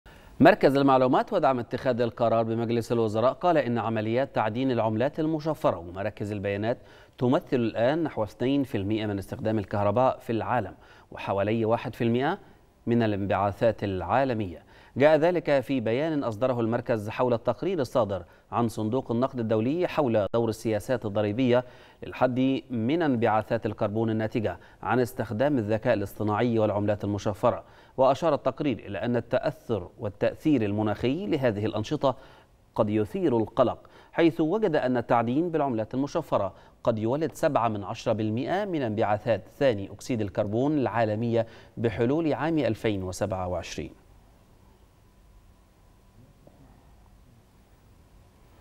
مركز المعلومات: عمليات تعدين العملات المشفرة ومراكز البيانات تمثل 2% من استخدام الكهرباء وحوالي 1٪
مركز المعلومات: عمليات تعدين العملات المشفرة ومراكز البيانات تمثل 2% من استخدام الكهرباء وحوالي 1٪ من الانبعاثات العالمية